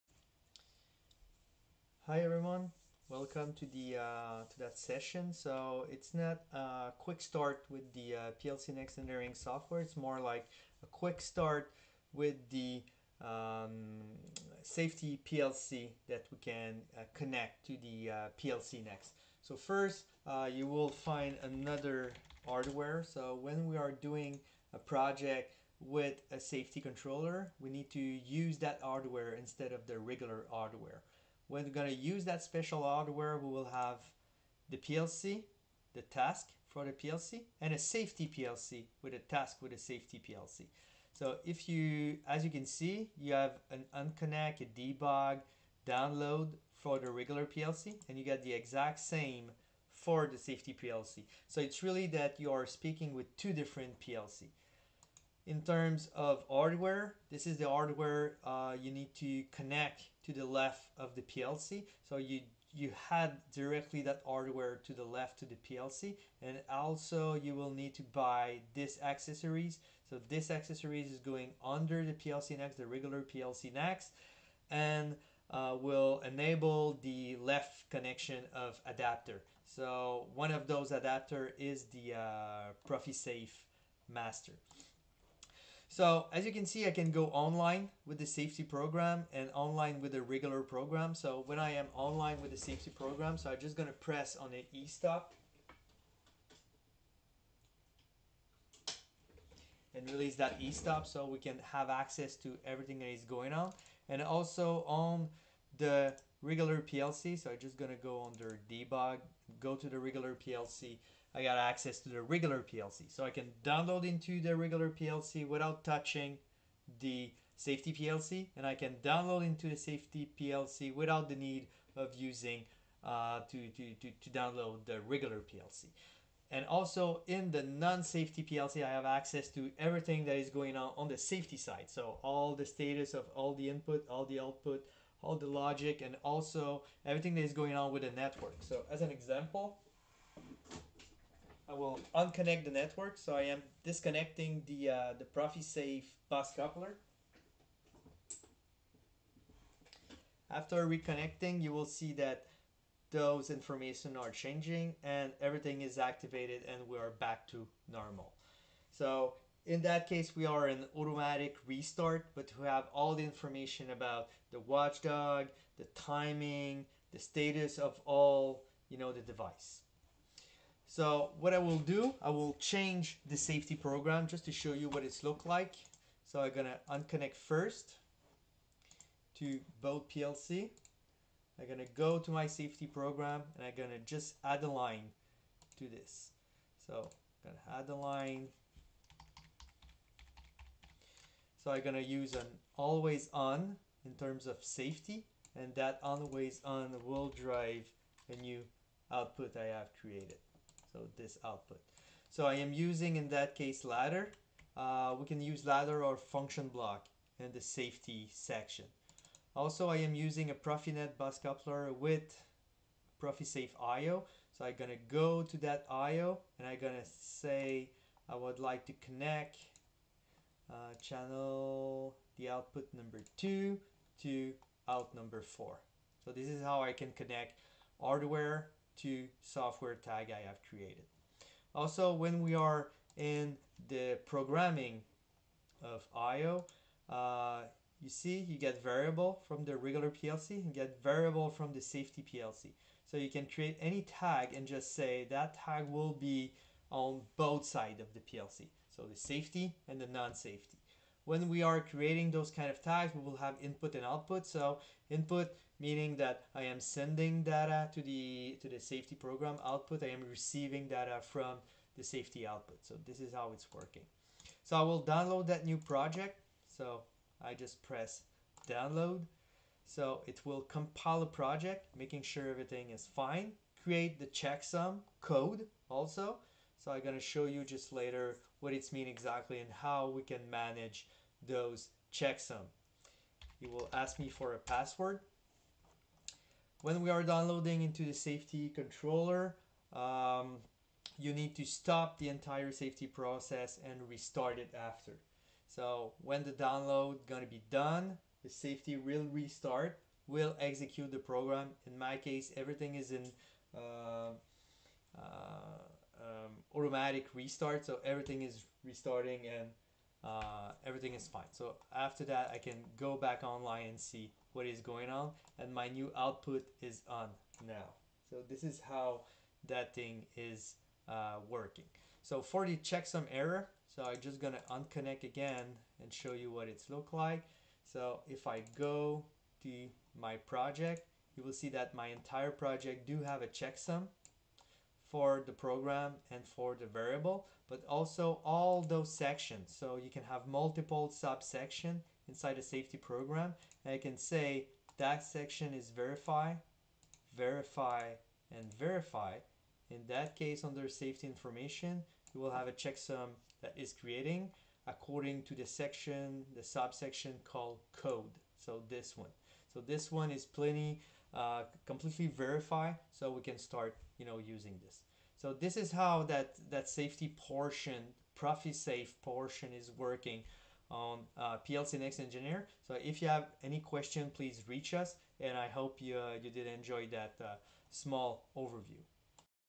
SPLC Overview, PLCnext Engineer, Phoenix Contact
Overview of the SPLC with the AXC F 2152, Profisafe Controller From Phoenix Contact